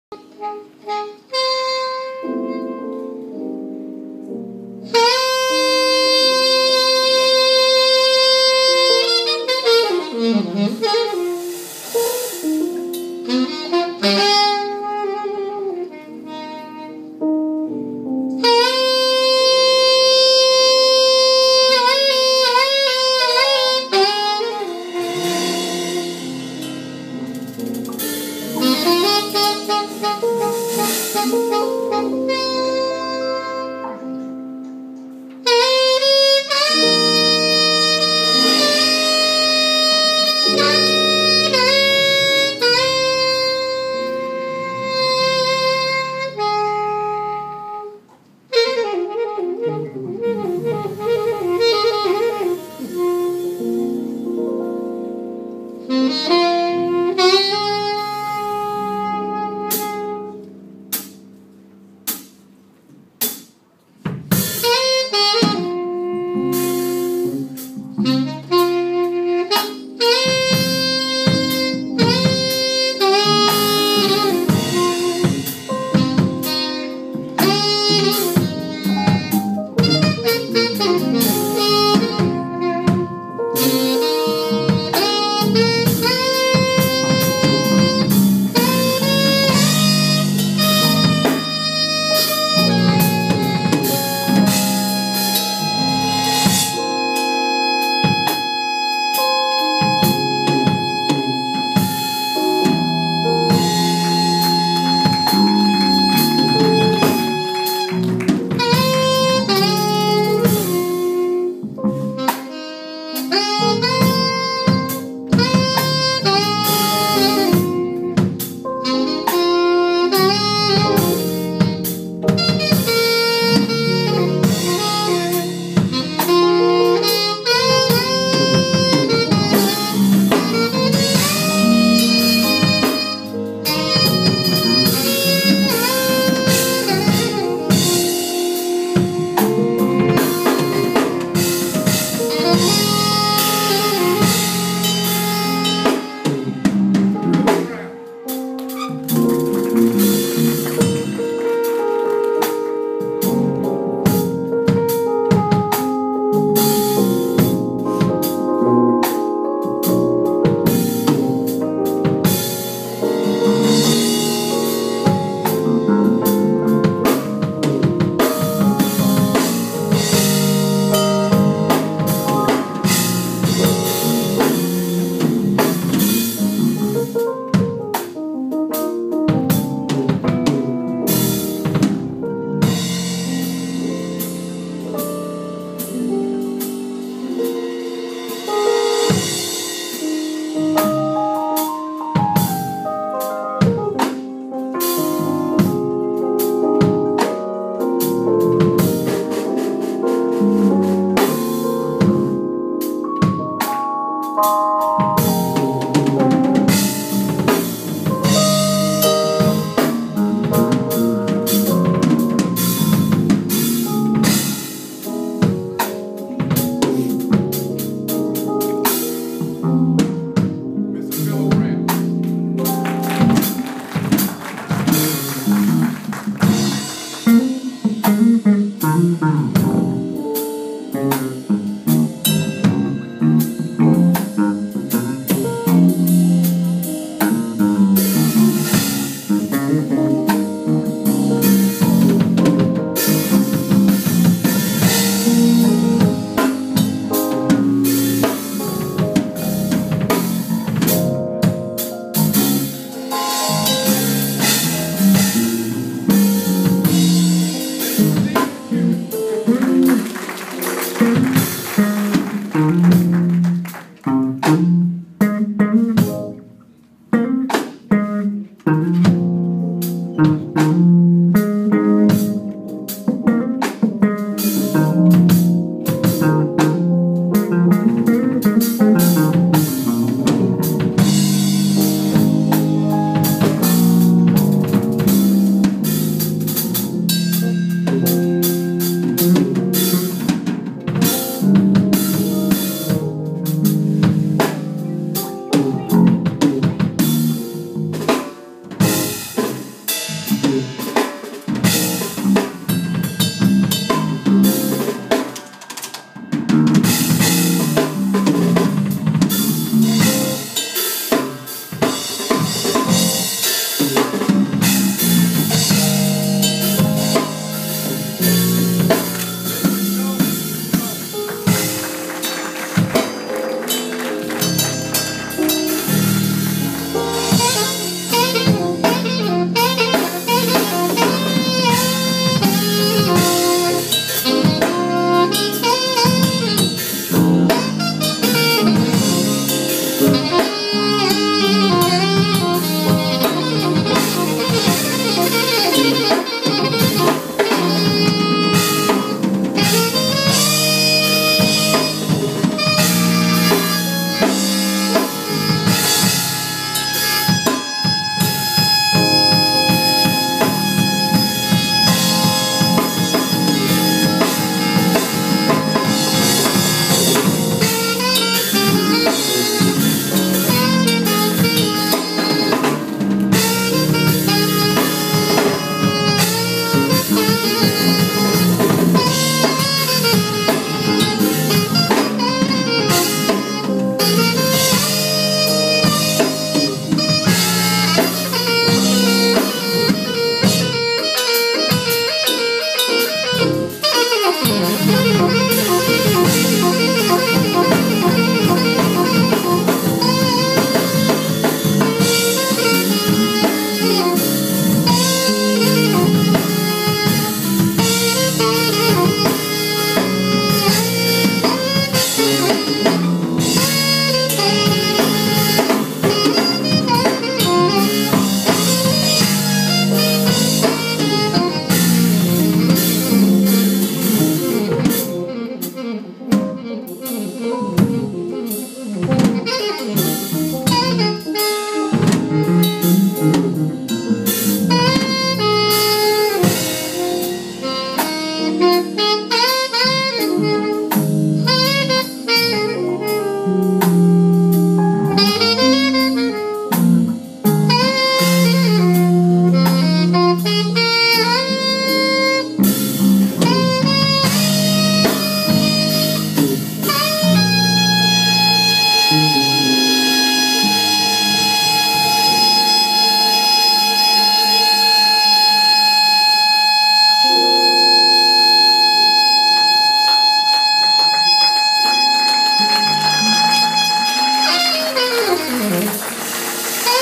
Phillip Brown and William Thornton
Good friends playing for A phi A black history program feb 2012